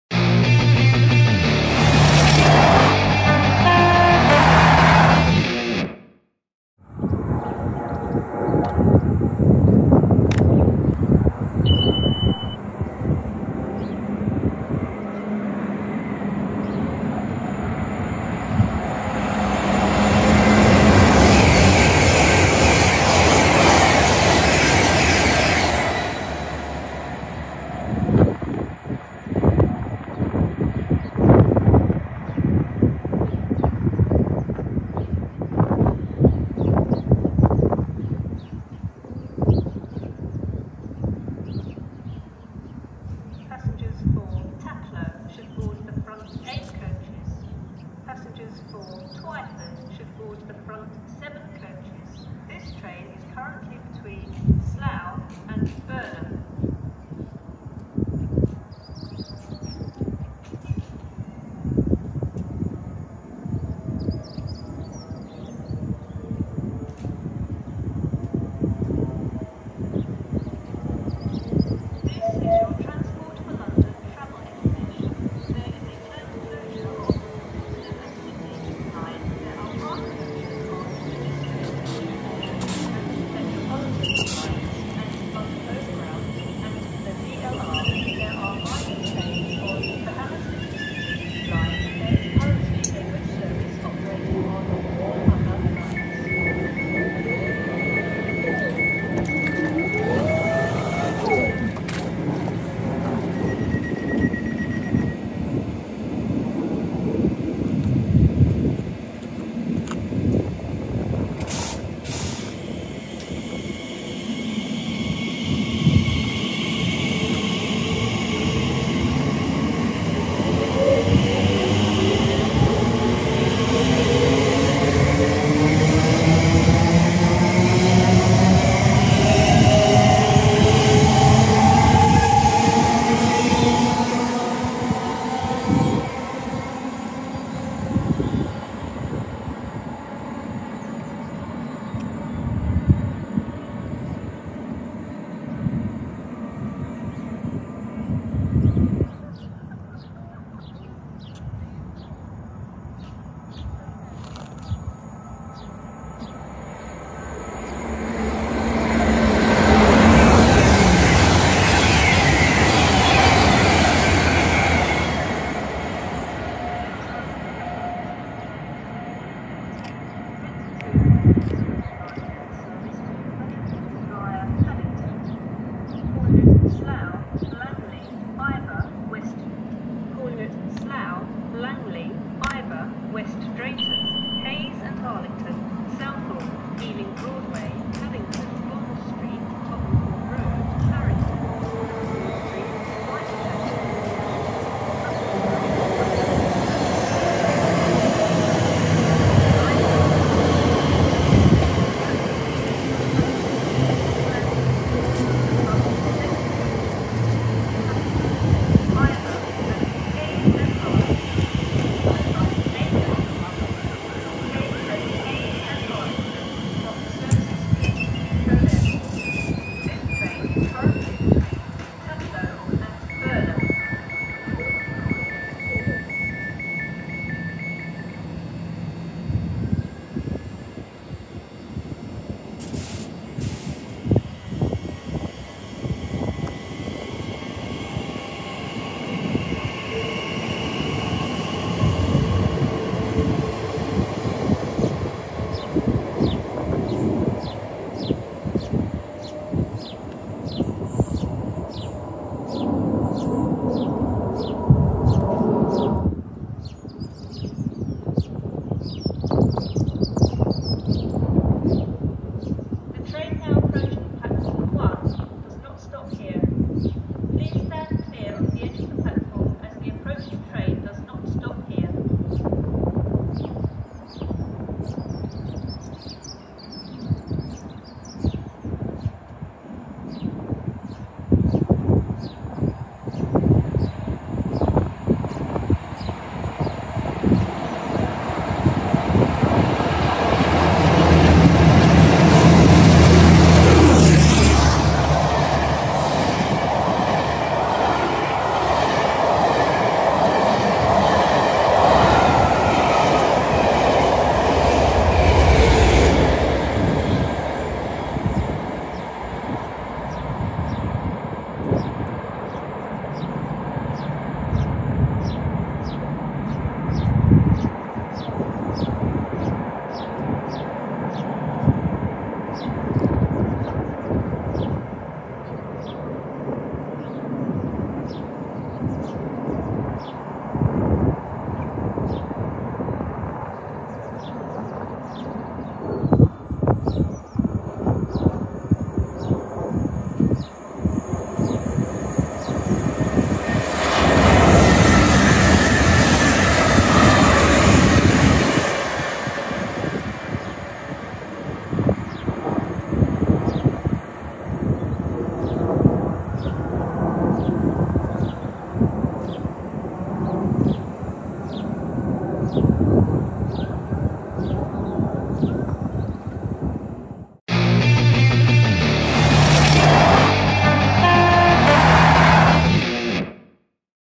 Trains at Burnham 140424 4K
Sunday afternoon at Burnham on the GWML with services to and from London Paddington.  Trains from GWR, Elizabeth Line, a railtour with D1015 Western Champion